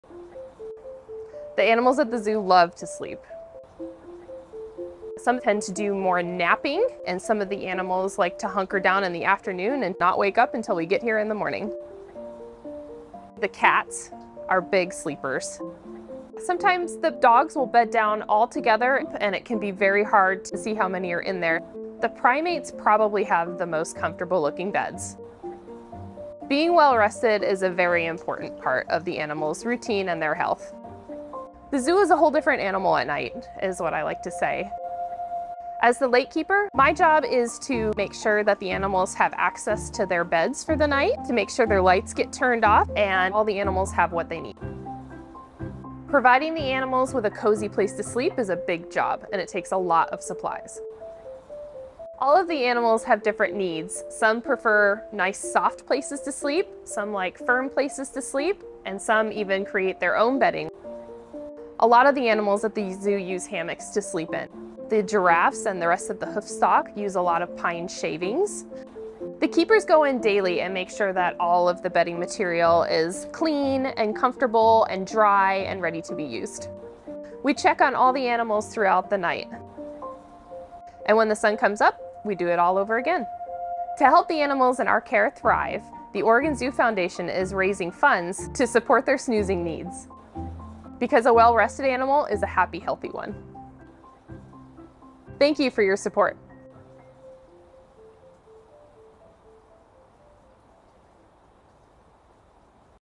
Celebrate Giving Snoozeday at the Oregon Zoo
You make this possible! This GivingTuesday, help keep the animals cozy with a gift to the Oregon Zoo Foundation. Thank you!

Whether it's enough for one bale of hay, a soft blanket or a custom-made black bear hammock — make every snooze a cozy one!